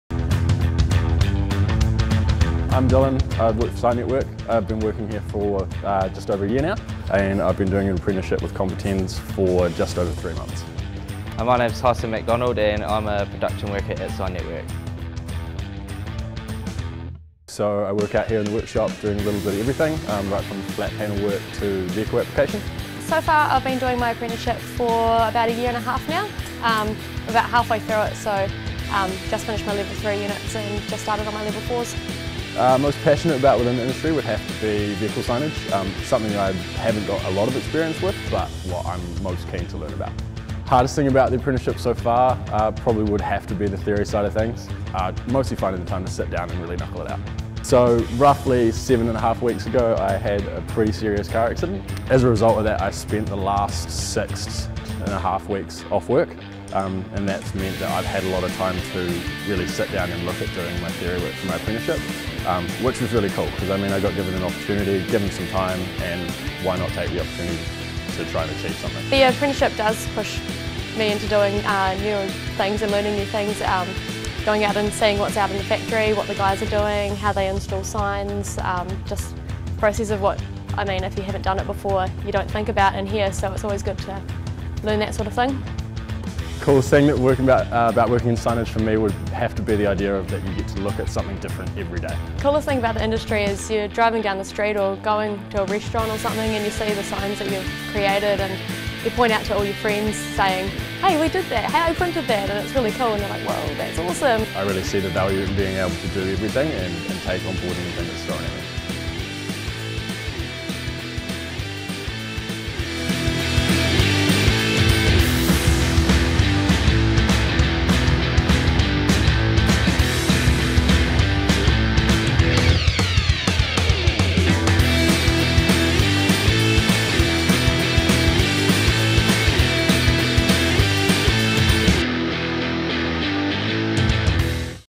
Competenz Vehicle Graphics with the Sign Network Apprentices
Check out our Apprentices in action putting together these vehicle graphics for Competenz